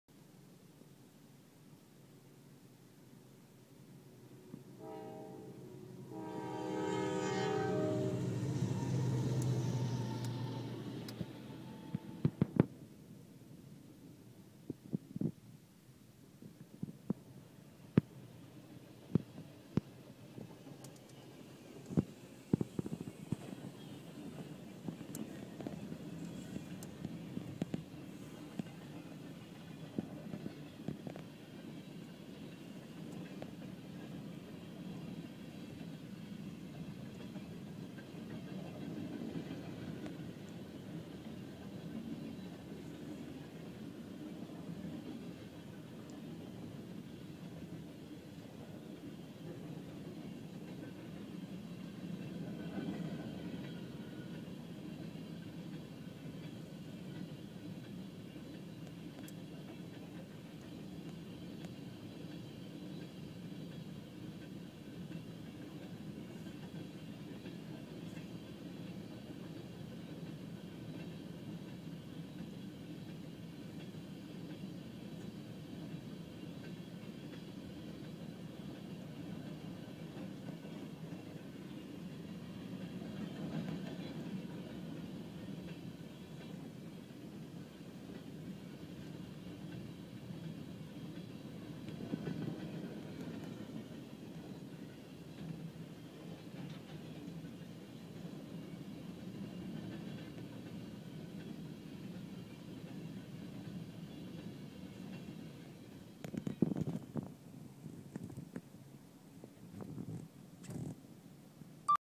NS 309 train with BNSF power
This is NS 309 train with one BNSF engine